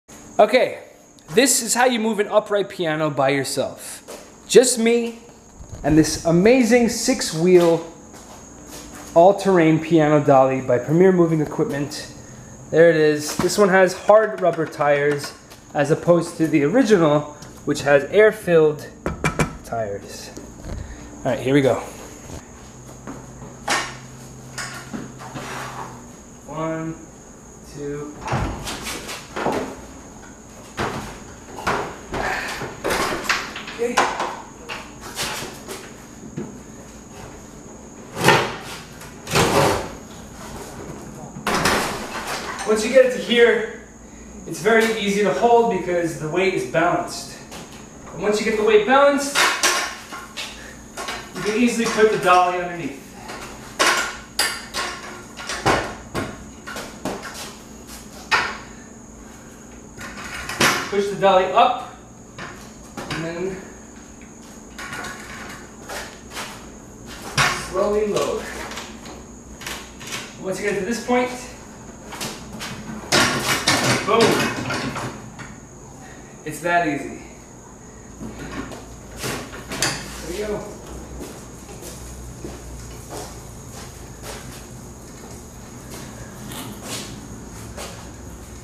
How to Move a 500 pound piano BY YOURSELF!
The All-Terrain Piano Dolly by Premiere Moving Equipment is located here. Tell them I sent you!